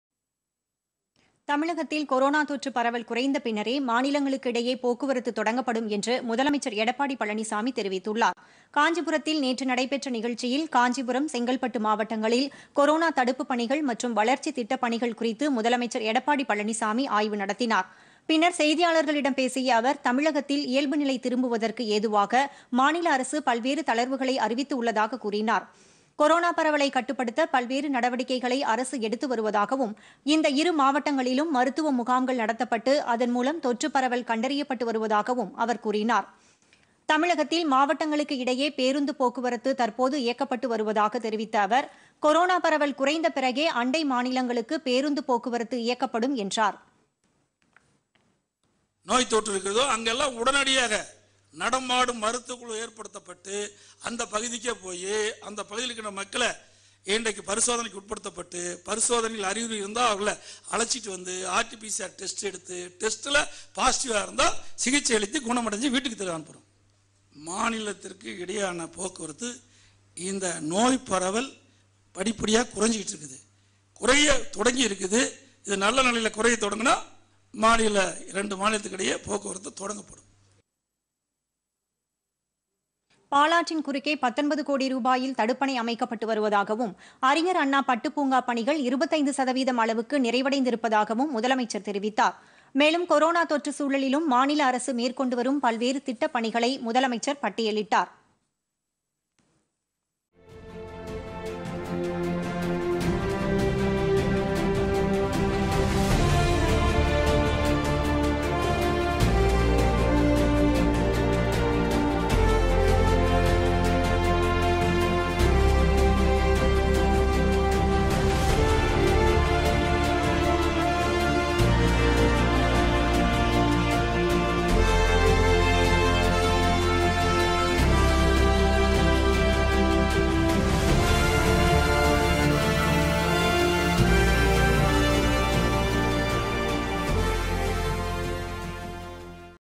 தொற்று குறைந்த பிறகே மாநிலங்களிடையே போக்குவரத்து
தமிழகத்தில் கொரோனா தொற்று பரவல் குறைந்த பின்னரே, மாநிலங்களுக்கிடையே போக்குவரத்து தொடங்கப்படும் - முதலமைச்சர் எடப்பாடி பழனிசாமி பேட்டி